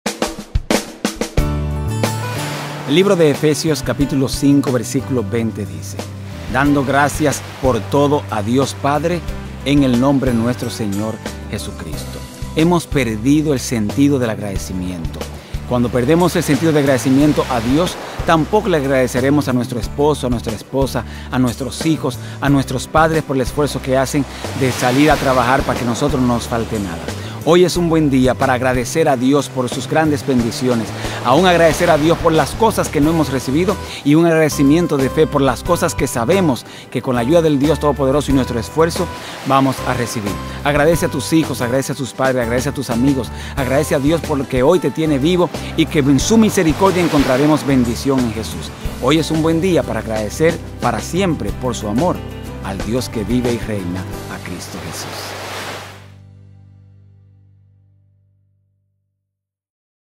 HED71 Agradece
Hemos perdido el sentido del agradecimiento. Debemos agradecer a Dios aun por las cosas que no hemos recibido. Solo agradece. Basado en Efesios 5:20